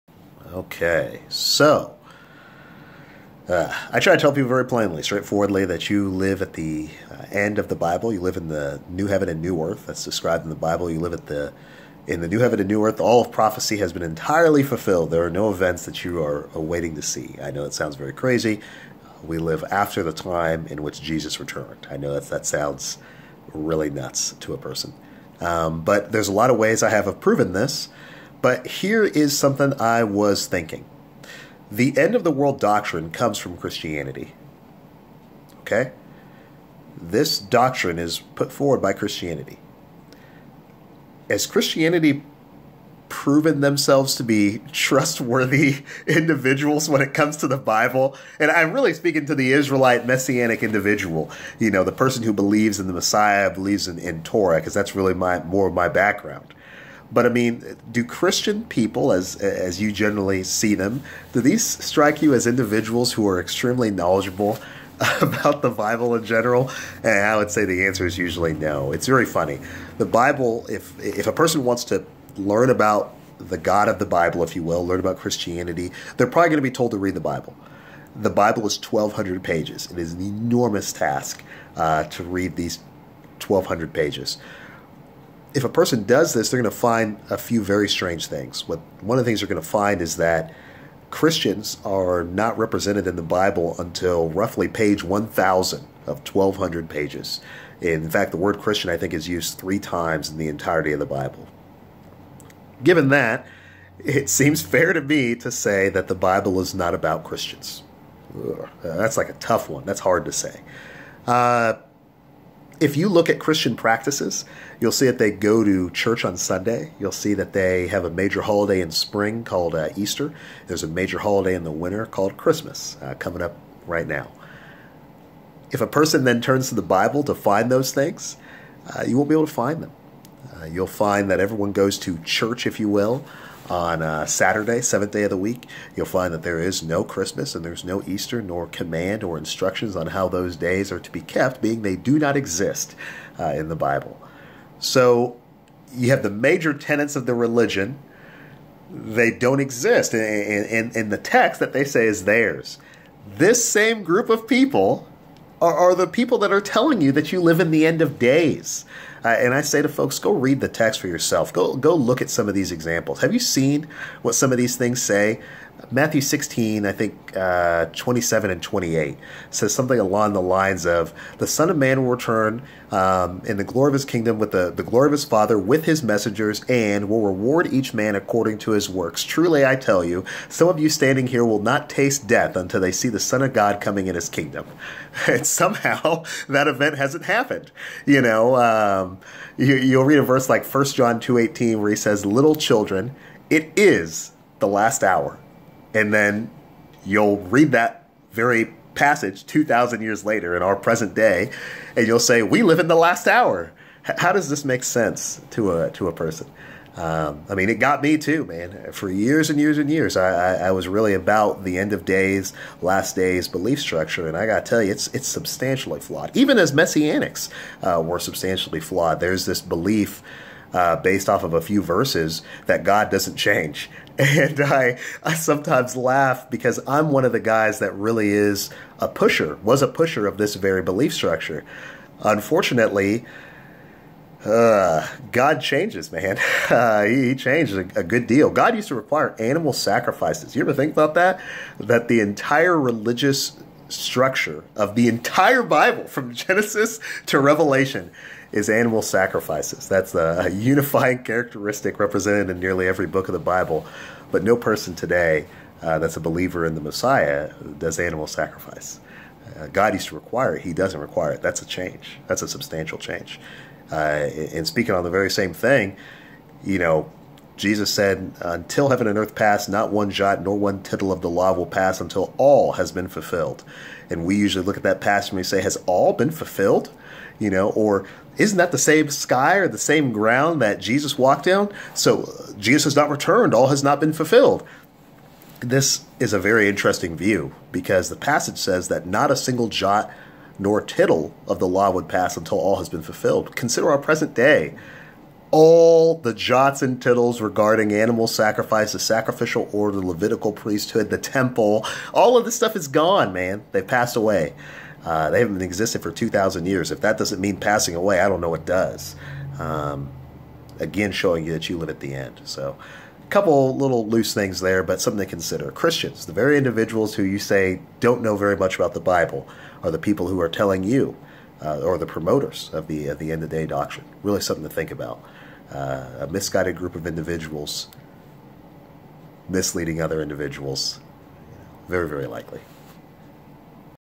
Christianity and the End of Days Tradition/Doctrine
Believing that you live in the end of days is Christian tradition.  The end was 2000 years ago.